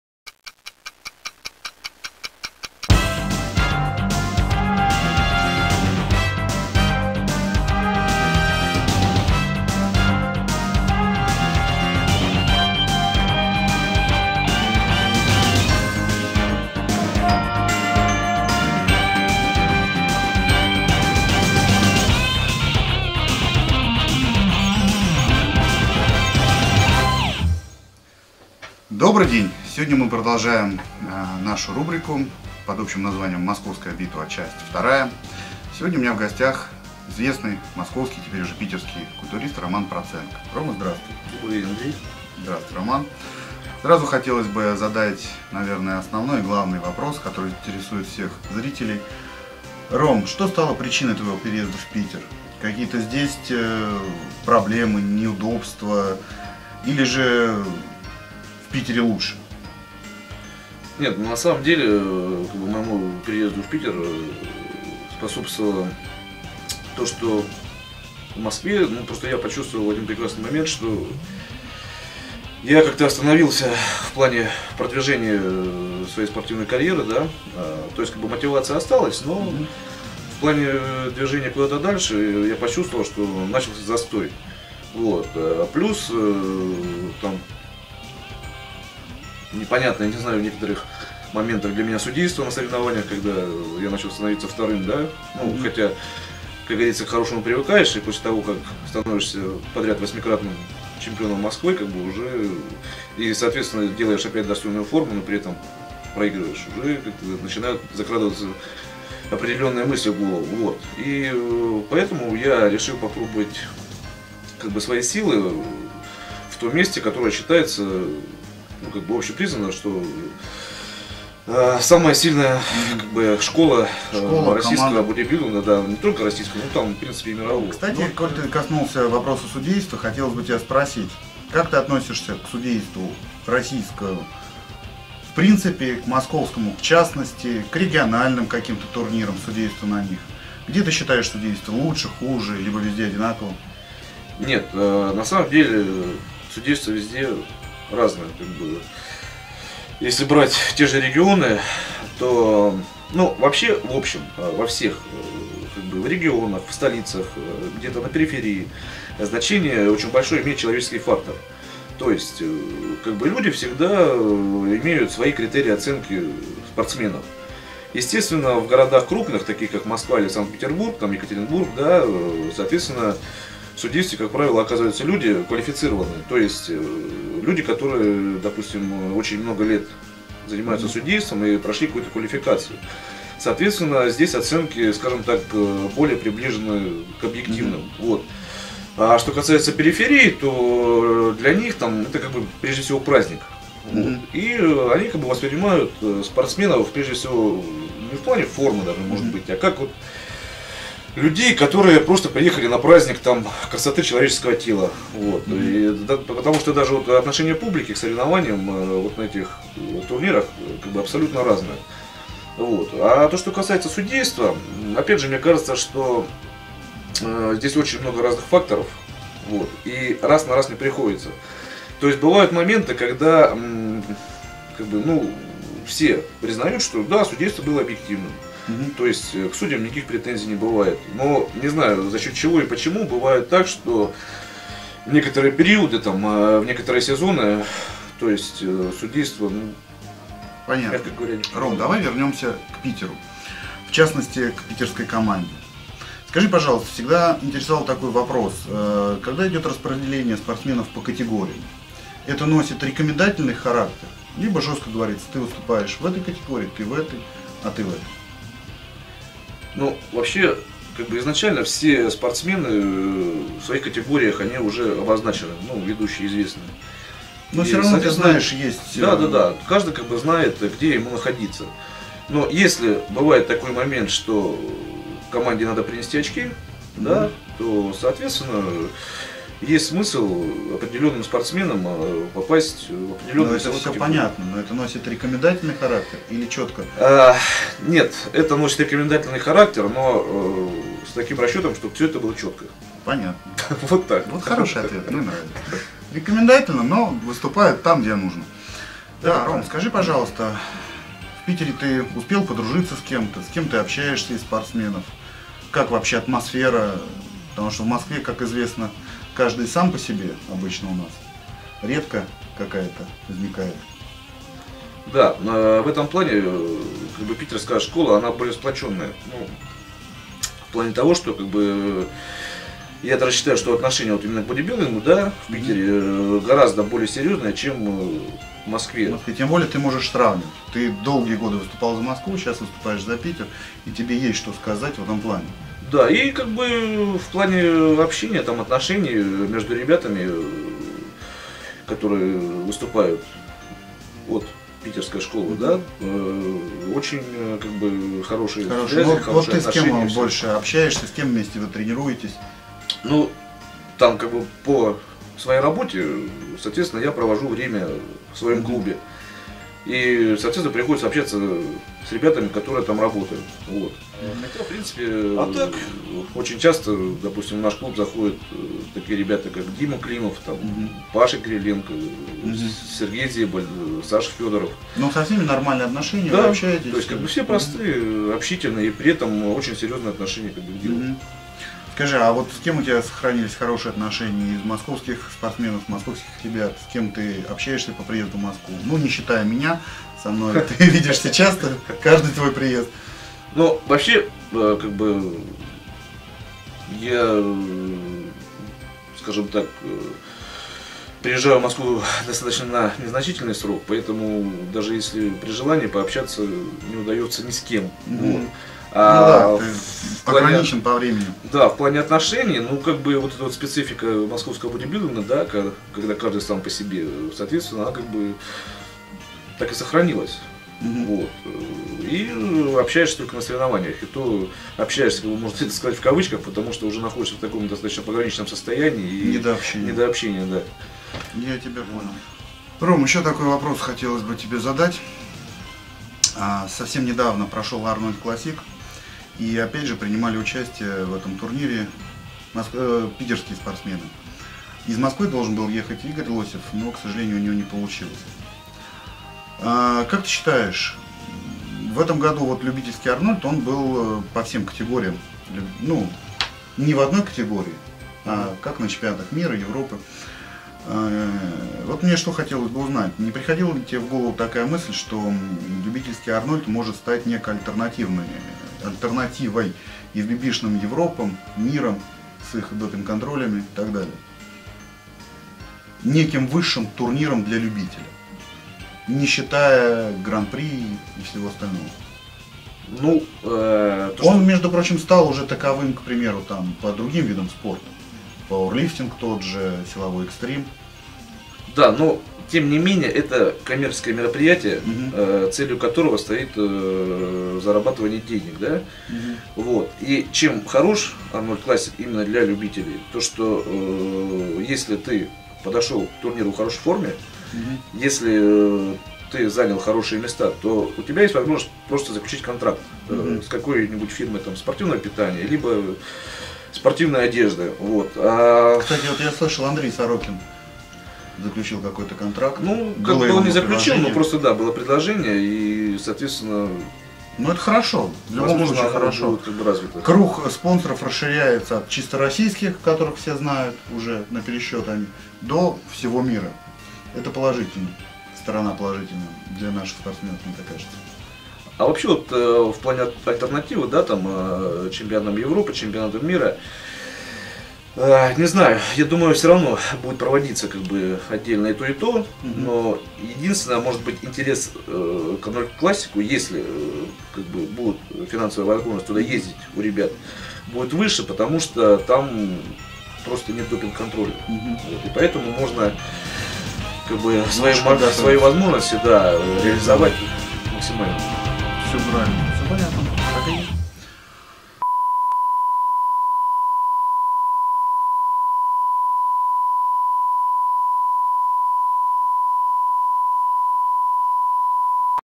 Весна 2010 Роман Проценко (Roman Protsenko) FITSPORT.RU Часть 1
«Московская Битва-2» Весна 2010 Интервью Романа Проценко (Roman Protsenko)  для FITSPORT.RU 15.03.10 Часть 1